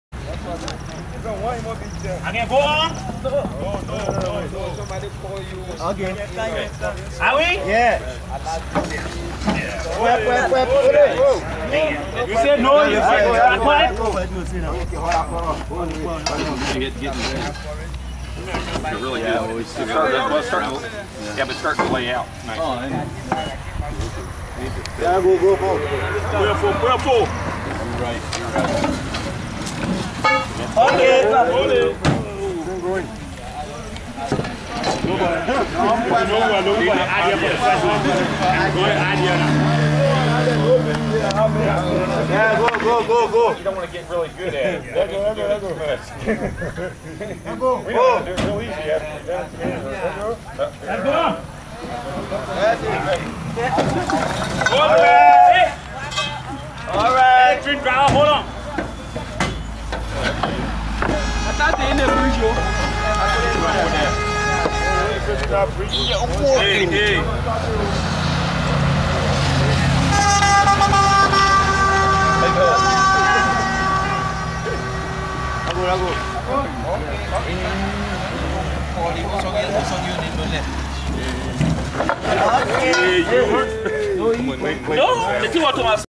Moving generators.
The generators that will power the wood shop were delivered to the roadside and then had to be moved by hand to their place in the new shop.